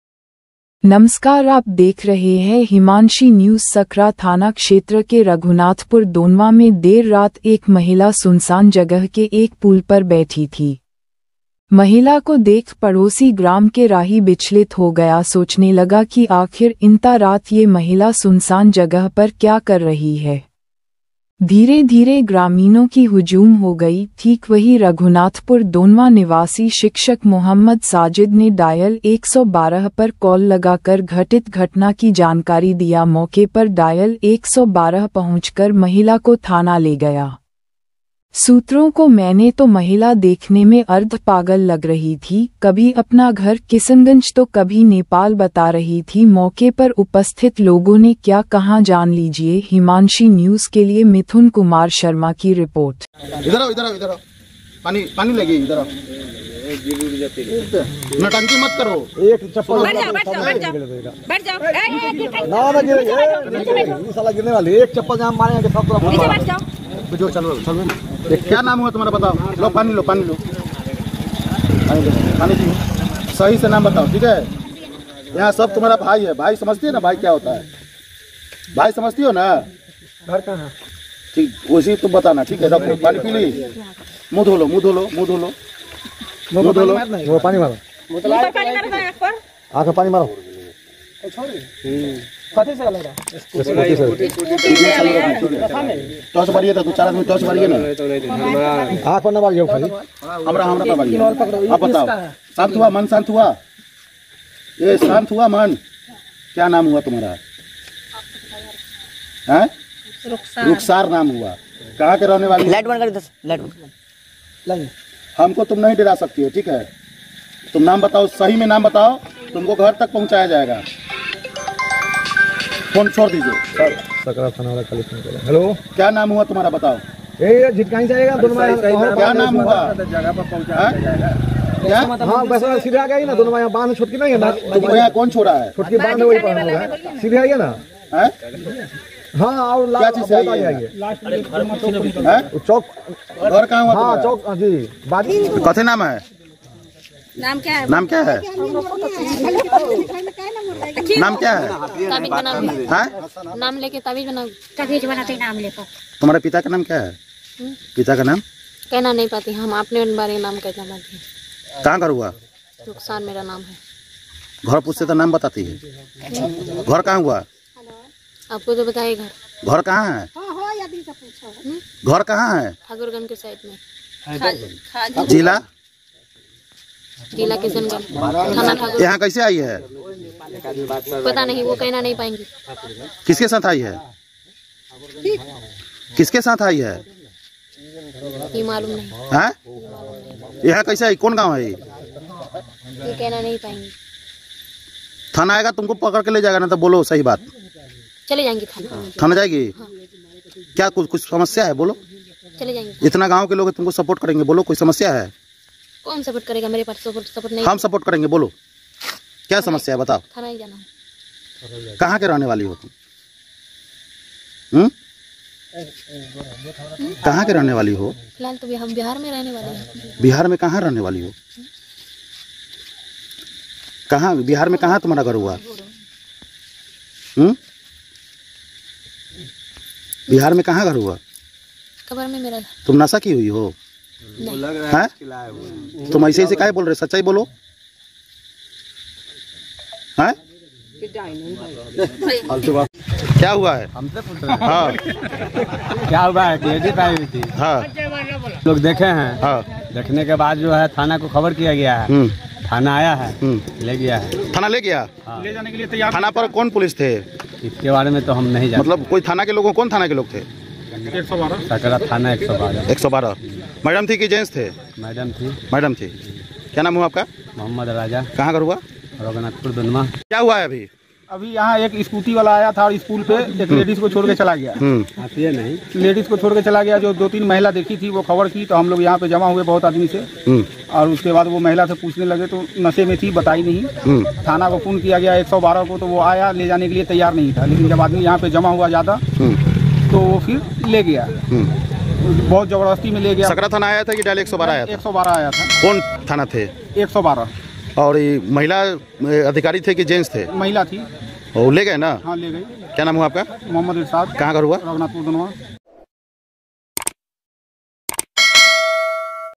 सकरा के रघुनाथपुर दोनवां में देर रात अज्ञात महिला को देख हैरान लोग, फिर महिला को थाना को सौपा गया
सकरा थाना क्षेत्र के रघुनाथपुर दोनवां में देर रात एक महिला सुनसान जगह के एक पूल पर बैठी थी, महिला को देख पड़ोसी ग्राम के राही बिचलित हो गया सोचने लगा की आखिर इनता रात ये महिला सुनसान जगह पर क्या कर रही है, धीरे धीरे ग्रामीणों की हुजूम हो गई ठीक वही रघुनाथपुर दोनवां निवासी शिक्षक मोहम्मद साजिद ने दयाल 112 पर कॉल लगा कर घटित घटना की जानकारी दिया मौके पर डायल 112 पहुंच कर महिला को थाना ले गया ! सूत्रों को मैने तो महिला देखने में अर्ध पागल लग रही थी कभी अपना घर किसनगंज तो कभी नेपाल बता रही थी  !

मौके पर उपस्थित लोगो ने क्या कहाँ जान लीजिए

घटना दिनांक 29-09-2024, समय 10:30Pm